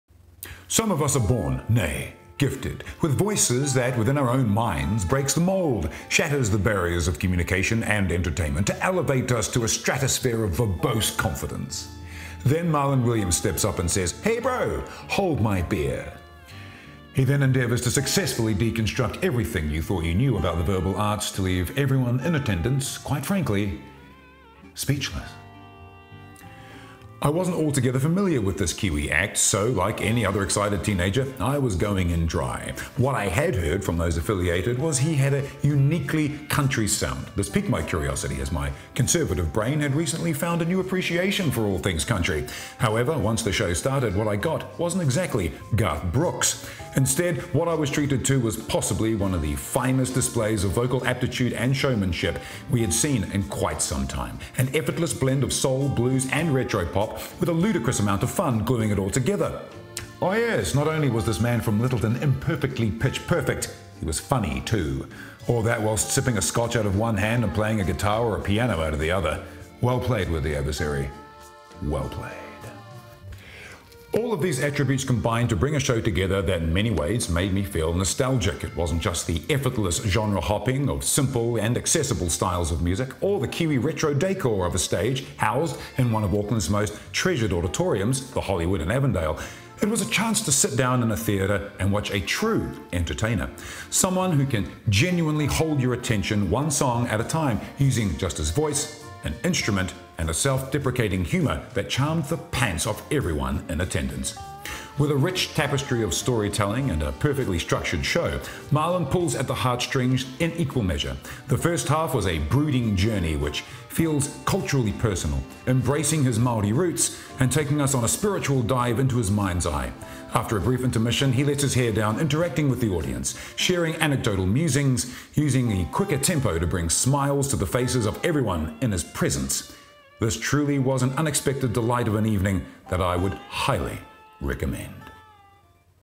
Marlon Williams - Auckland Concert Review (Full Concert Review)
Marlon Williams Concert Review by Daryl Habraken
Concert Details:The Hollywood Theatre, Auckland, NZL25th March 2021
Ft. Photography by Doug Peters (Ambient Light NZ)